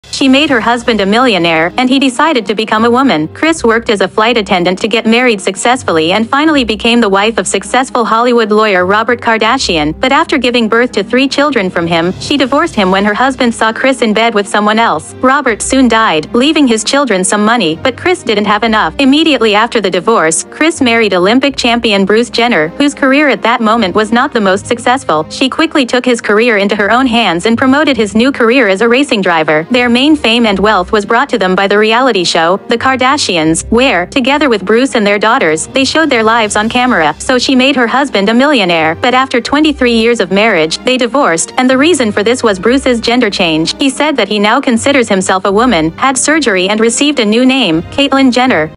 Kris Jenner, Robert Kardashian and Bruce Jenner/
She made her husband a millionaire, and he decided to become a woman. Kris worked as a flight attendant to get married successfully and finally became the wife of successful Hollywood lawyer Robert Kardashian, but after giving birth to three children from him, she divorced him when her husband saw Kris in bed with someone else. Robert soon died, leaving his children some money, but Chris didn't have enough. Immediately after the divorce, Kris married Olympic champion Bruce Jenner, whose career at that moment was not the most successful. She quickly took his career into her own hands and promoted his new career as a racing driver. Their main fame and wealth was brought to them by the reality show “The Kardashians,” where, together with Bruce and their daughters, they showed their lives on camera. So she made her husband a millionaire. But after 23 years of marriage, they divorced, and the reason for this was Bruce's gender change. He said that he now considers himself a woman, had surgery and received a new name: Caitlyn Jenner.